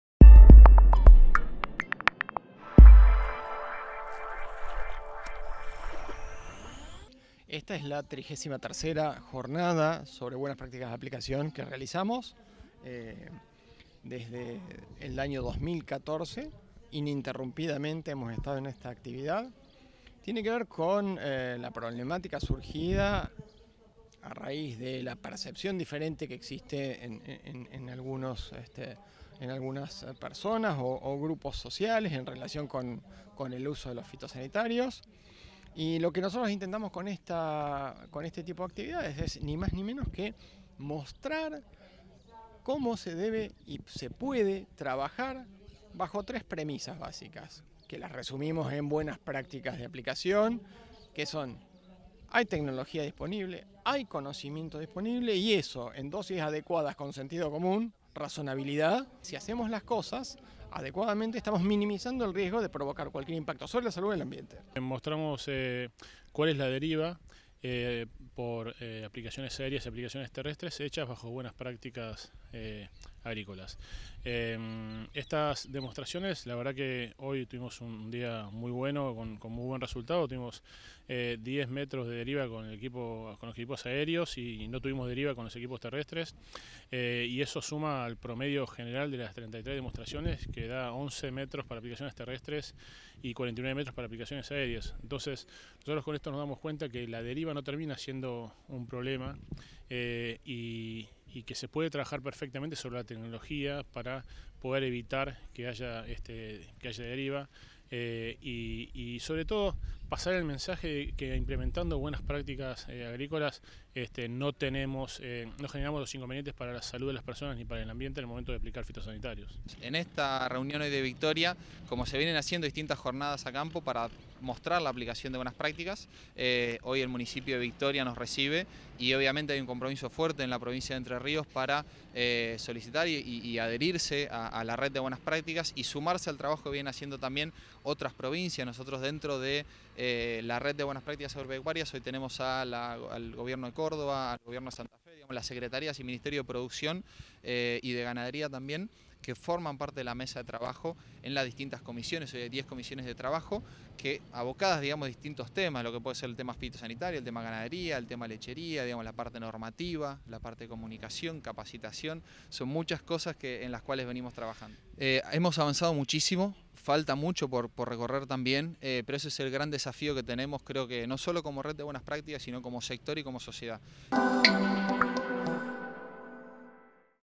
Demostración de Buenas Prácticas Agrícolas
En la localidad de Victoria, Entre Ríos, el secretario de Gobierno de Agroindustria, Luis Miguel Etchevehere, presentó el informe final del grupo de trabajo sobre Buenas Prácticas en materia de aplicaciones fitosanitarias, integrado por expertos de las Secretarías de Agroindustria; Ambiente y Desarrollo Sustentable; Salud; Ciencia y Tecnología, además del Senasa y el INTA, y representantes del Consejo Federal del Medio Ambiente (COFEMA) y el Consejo Federal Agropecuario (CFA).
Asimismo, se realizó una demostración de Buenas Prácticas Agrícolas (BPA) que incluyó el uso correcto de envases a través del triple lavado y el lavado a presión realizado por la Cámara de Sanidad Agropecuaria y Fertilizantes (CASAFE) y la Secretaría de Agroindustria. Además de una muestra aérea y terrestre del uso correcto de fitosanitarios en campo.